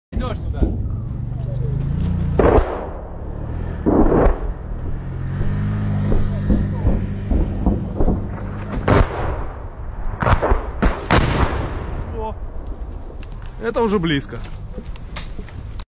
Ukraine War: Russian Grad Rocket Artillery Barrage Misses Ukrainian Marines by a Few Hundred Meters
I've said it before and I'll say it again...
I'd rather be lucky, than smart...